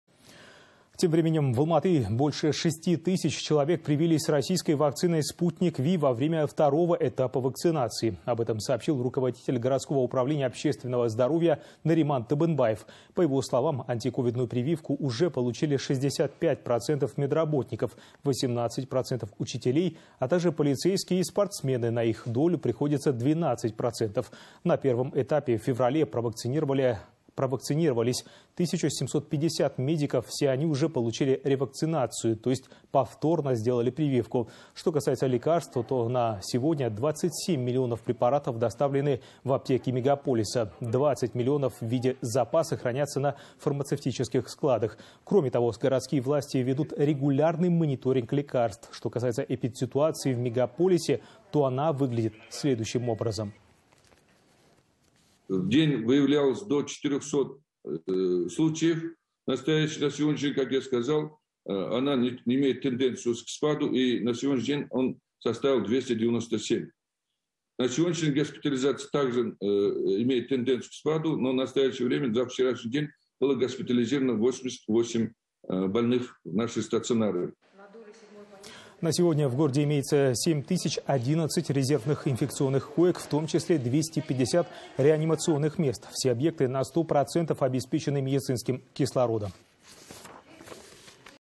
Второй этап массовой вакцинации: больше 6000 человек привились "Спутником-V" в Алматы (16.03.21)
В Алматы больше 6000 человек привились российской вакциной "Спутник-V" во время второго этапа вакцинации. Об этом сообщил руководитель городского Управления общественного здоровья Нариман Табынбаев. По его словам, антиковидную прививку уже получили 65% медработников, 18% учителей, а также полицейские и спортсмены, на их долю приходится 12%, передает корреспондент Almaty.tv.

На первом этапе в феврале провакцинировались 1 750 медиков, все они уже получили ревакцинацию, то есть повторно сделали прививку. Что касается лекарств, то на сегодня 27 миллионов препаратов доставлены в аптеки мегаполиса, 20 миллионов в виде запаса хранятся на фармацевтических складах. Кроме того, городские власти ведут регулярный мониторинг лекарств.

"В день выявлялось до 400 случаев. В настоящее время она не имеет тенденции к спаду. И на сегодня этот показатель составил 297. Госпитализация имеет тенденции к спаду, но за вчерашний день было госпитализировано 88 больных в наши стационары", - сообщил Нариман Табынбаев.

На сегодня в городе имеется 7 011 резервных инфекционных коек, в том числе 250 реанимационных мест. Все объекты на 100% обеспечены медицинским кислородом.